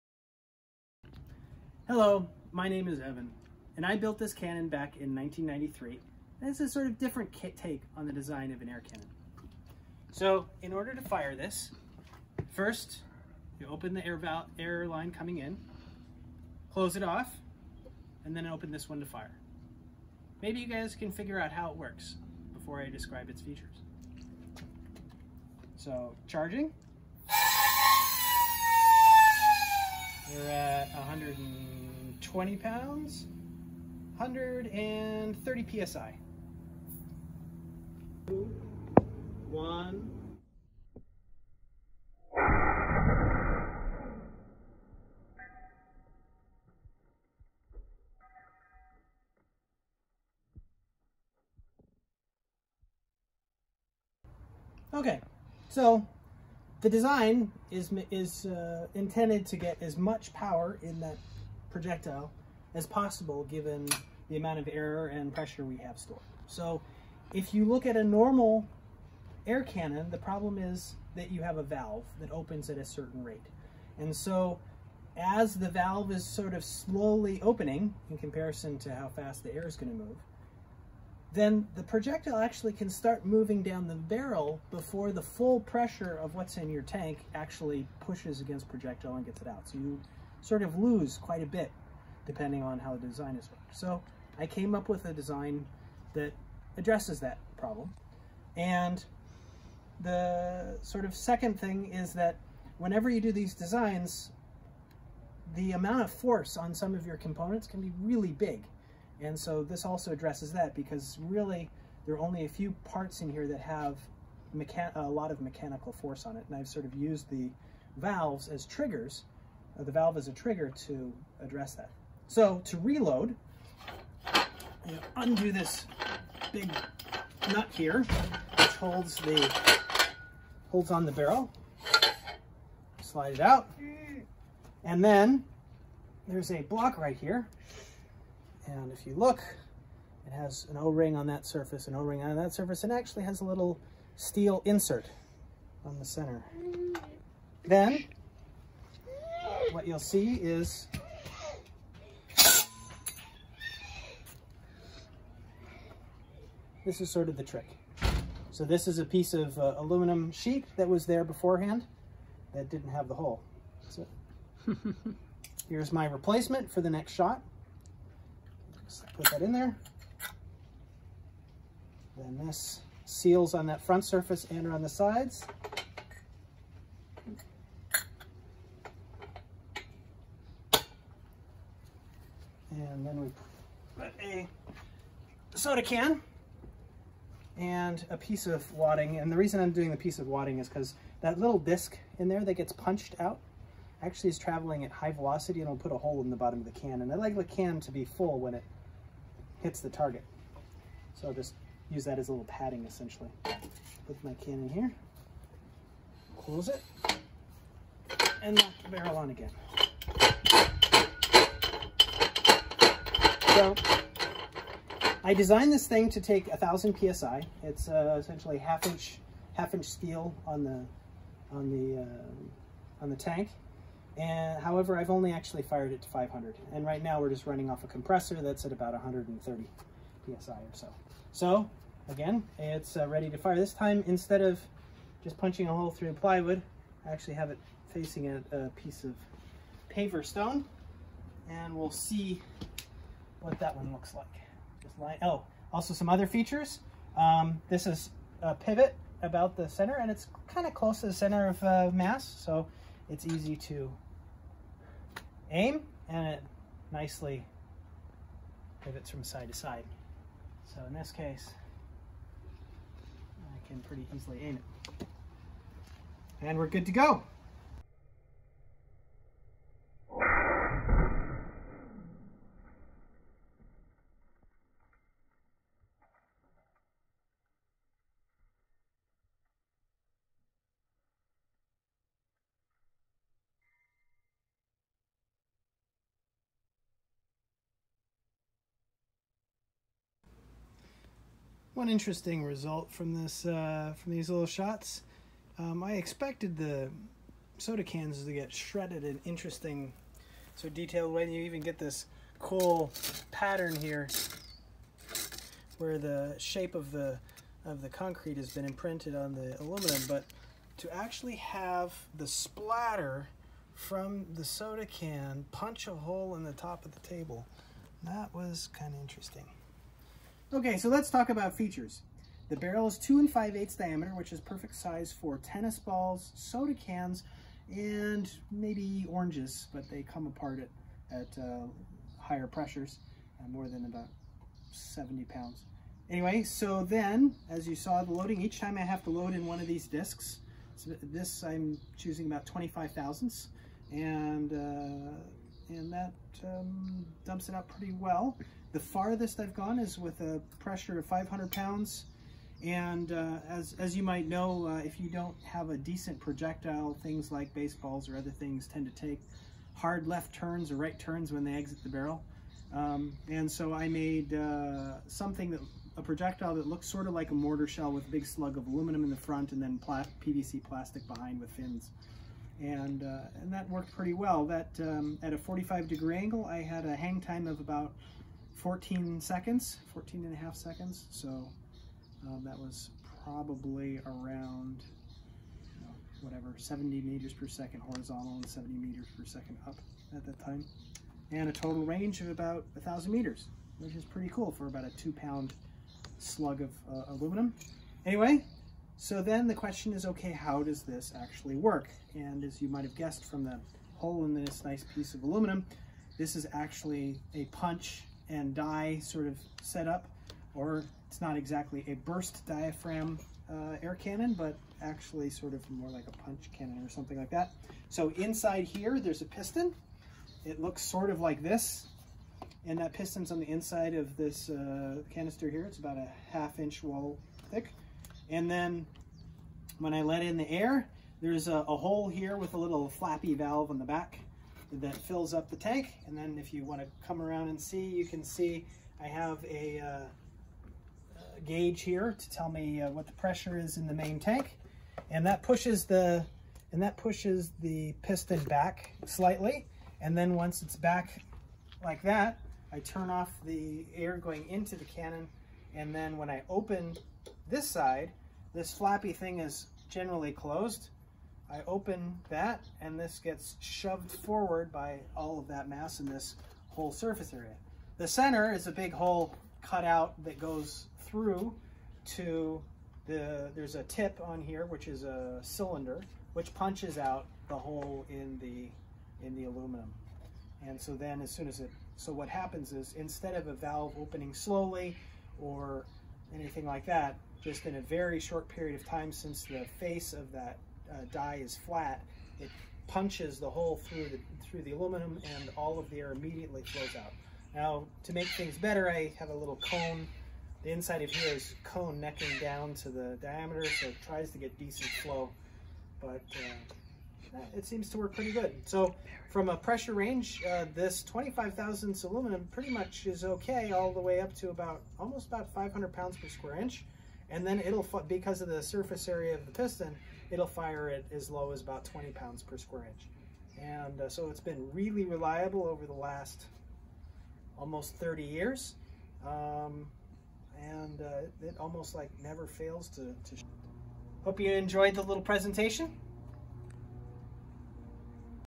A Different Air Cannon Design
This air cannon is very reliable and does not use a normal valve system.   It was designed to take up to 1000 psi, and has a barrel 5 ft long and 2-5/8" diameter.  At 500 Psi it fired a 2 lb. aerodynamic projectile about 1000 meters.

This is a rough first draft of the video, let me know if you have any suggestions.
I am thinking I could add addtional videos around this:
- higher pressure operation (500 psi - 1000 psi) How fast can it shoot without self distructing?
- Evacuated barrel - See how much difference this makes.
- A detailed design tradeoff video:  What really matters in an air cannon design ?
- High speed video : This would be really cool, but the camera rentals seem to be really expensive.
- re-do it to remove the crying child and wild parrots ?  :-)

I was inspired to do this video after watching Destin's video on Smarter Every Day where he shot a baseball at 1050 mph